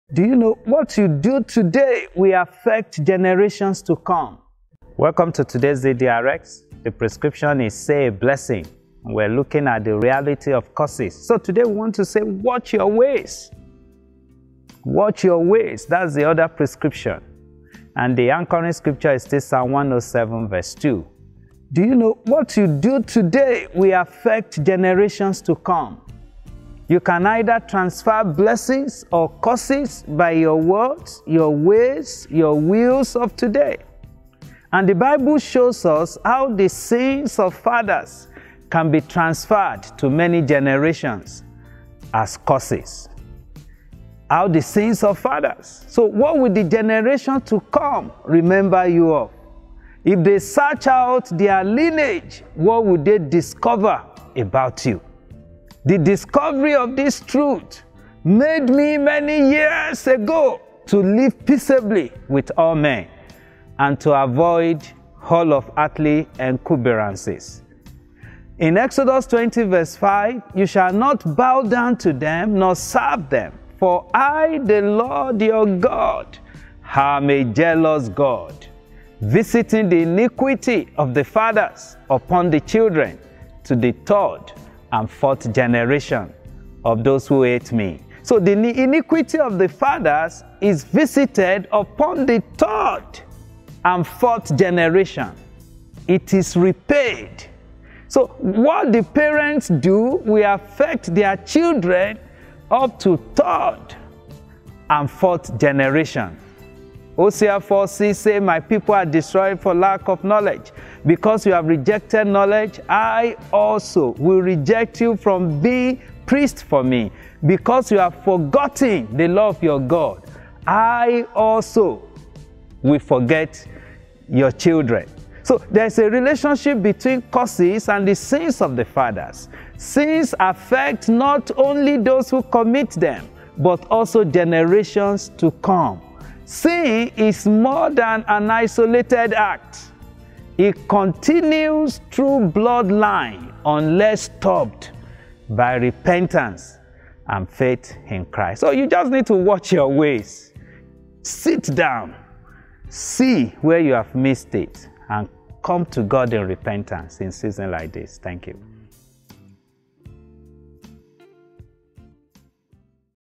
DDRxS34E03: Say A Blessing || Watch Your Ways
What you do today will affect generations to come so we need to be proactive in sowing good seeds today!

Stay in Touch with Us!